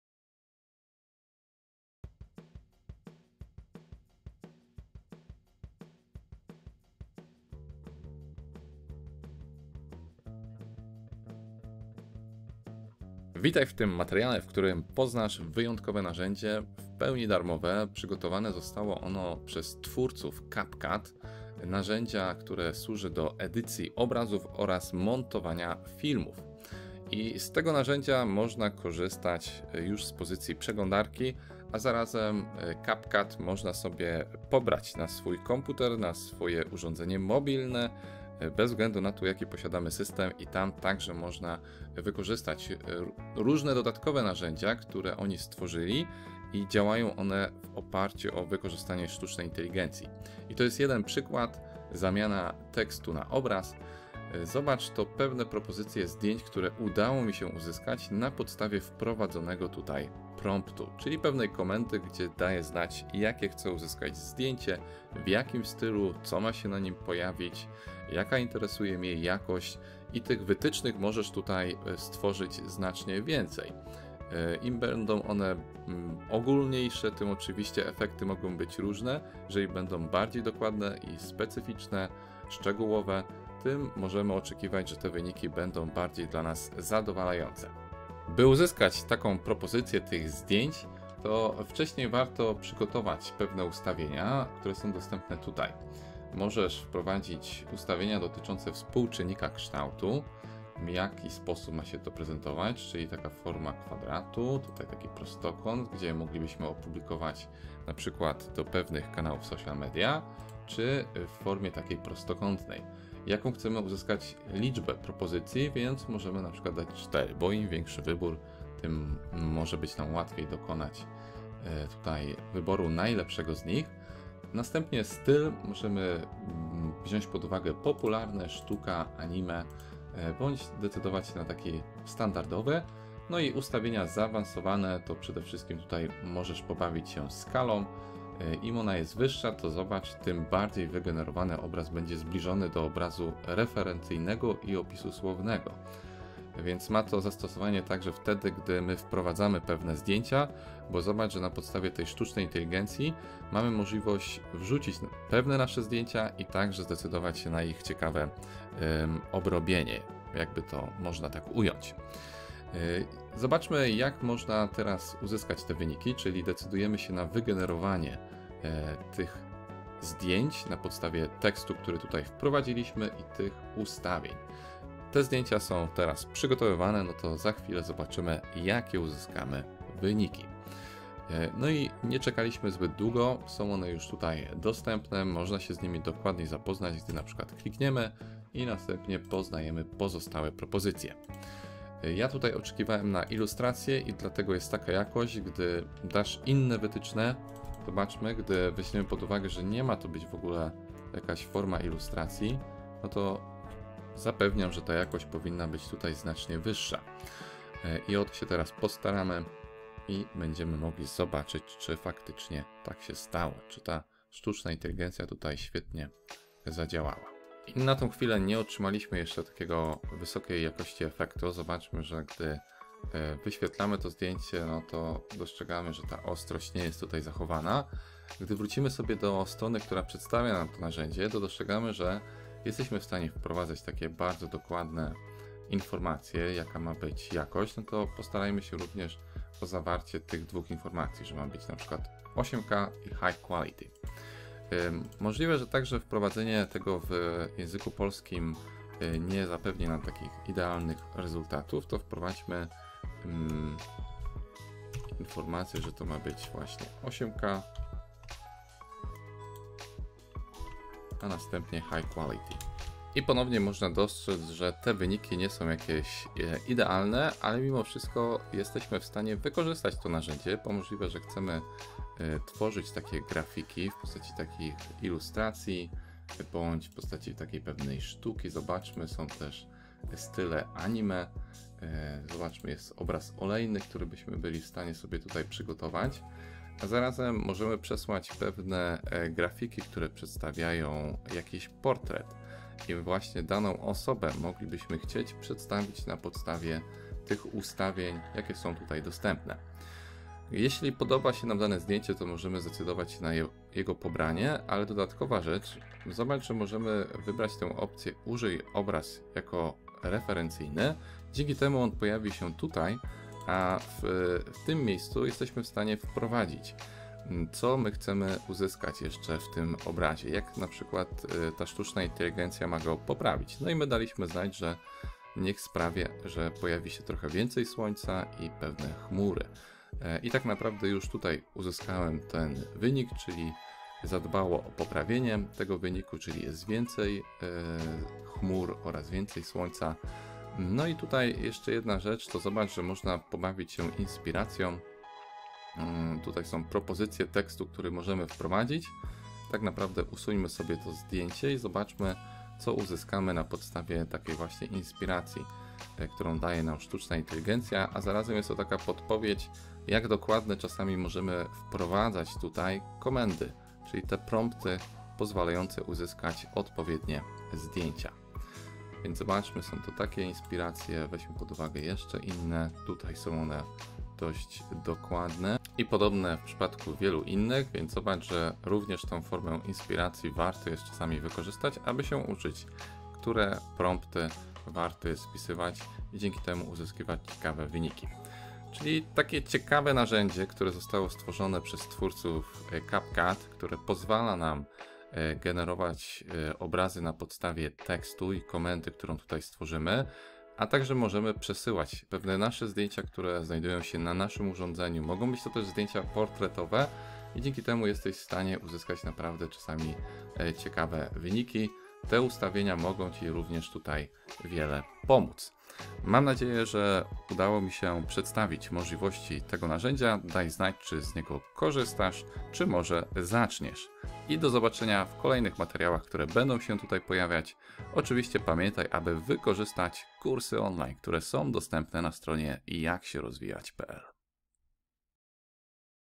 Narzędzie AI "Zamiana Tekstu Na Obraz" W CapCut 💻📷 Poznajemy I Testujemy ⭐
Odkryj rewolucję w edycji zdjęć i wideo z nowym narzędziem AI, które przekształca tekst na obrazy w CapCut - obejrzyj nasz film, aby zobaczyć, jak to działa w praktyce. Zobacz, jak łatwo możesz wzbogacić swoje projekty graficzne i wideo, korzystając z intuicyjnych i zaawansowanych funkcji AI 💻📷

🎥 ⏱️Spis treści filmu:⏱️🎥
00:00 - 00:14 Intro kanału Jak Się Rozwijać :)
00:15 - 00:50 Wstęp o możliwościach narzędzi AI od CupCut
00:51 - 03:00 Praktyczne zastosowanie aplikacji AI "zamiana tekstu na obraz" w CupCut
03:01 - 03:48 Wygenerowanie 4 zdjęć krajobrazowych za pomocą AI
03:49 - 04:46 Próba generowania zdjęć wyższej jakości
04:47 - 05:39 Przesyłanie własnych zdjęć i ich zastosowanie w narzędziu AI
05:40 - 06:28 Stosowanie lepszych promptów / komend w aplikacji "zamiana tekstu na obraz"
06:29 Podsumowanie możliwości aplikacji AI od CapCut "zamiana tekstu na obraz".

🔥 NARZĘDZIA DO BIZNESU ONLINE + ZNIŻKI: 🔥

🛟 W CZYM MOŻEMY CI POMÓC? 👍